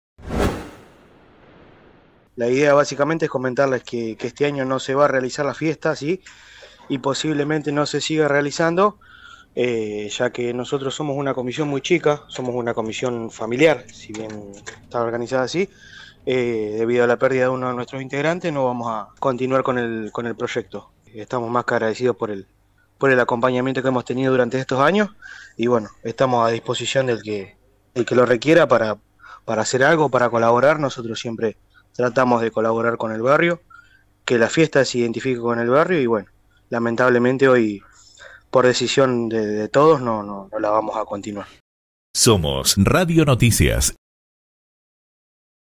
No se realizará la Fiesta de la Empanada Costera
Comunicación con Facundo Broglio, integrante de la comisión organizadora.